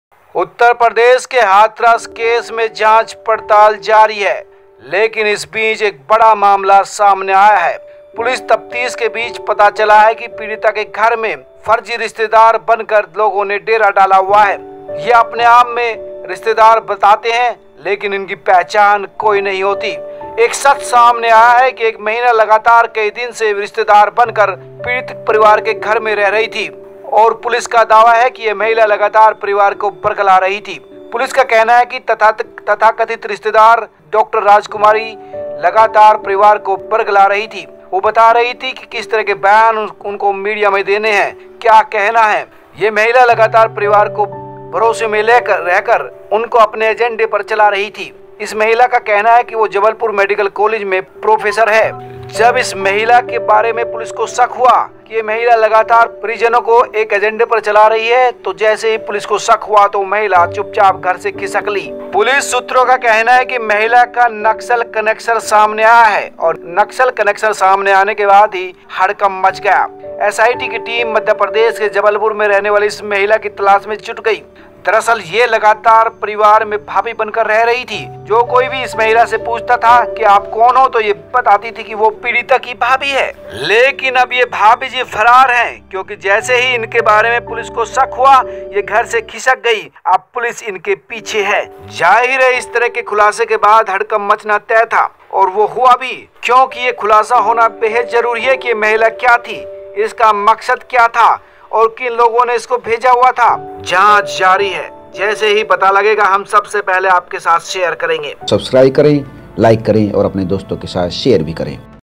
Hathras,भाभी बनकर पीडित परिवार को फंसा रही थी नक्सली महिला.परिवार को बरगला कर दिला रही थी बयान।
उत्तर प्रदेश में हाथरस मामले को लेकर जांच पड़ताल जारी है. इस बीच, तफ्तीश में पता चला है कि पीड़िता के गांव में फर्जी रिश्तेदारों ने डेरा डाला हुआ था. अपने आप को रिश्तेदार बता रही महिला का सामने सच आया है.
उत्तर प्रदेश में हाथरस गैंगरेप मामले को लेकर जांच पड़ताल जारी है. इस बीच, तफ्तीश में पता चला है कि पीड़िता के गांव में फर्जी रिश्तेदारों ने डेरा डाला हुआ था. अपने आप को रिश्तेदार बता रही महिला का सामने सच आया है. एक कथित महिला रिश्तेदार पीड़ित परिवार में देखी गई है.
पुलिस का दावा है कि वह महिला पीड़ित परिवार को बरगला रही थी. पुलिस के मुताबिक तथाकथित रिश्तेदार डॉ. राजकुमारी पीड़ित परिवारों को बरगलाते हुए देखी गई है. केवल दलित होने के नाते परिवार के लोगों को भरोसे में लेकर पिछले कई दिनों से पीड़ित परिवार के यहां महिला रही थी. महिला जबलपुर मेडिकल कॉलेज में अपने आप को प्रोफेसर बता रही थी.
पुलिस के मुताबिक यह फर्जी रिश्तेदार मीडिया में क्या बयान देना है, पीड़ित परिजनों को लगातार गाइड कर रही थी. पुलिस के शक होते ही महिला घर से चुपचाप खिसक ली. वीडियो में भी महिला को देखा जा सकता है जो अपना नाम कथित तौर राजकुमारी बता रही थी. Investigation is going on in the case of Hathras in Uttar Pradesh. Meanwhile, it is revealed in the investigation that fake relatives were camping in the victim's village. The truth of a woman telling herself a relative has come to the fore.
Investigation is going on in the case of Hathras gangrape in Uttar Pradesh. Meanwhile, it is revealed in the investigation that fake relatives were camping in the victim's village. The truth of a woman telling herself a relative has come to the fore. An alleged female relative has been seen in the victim's family.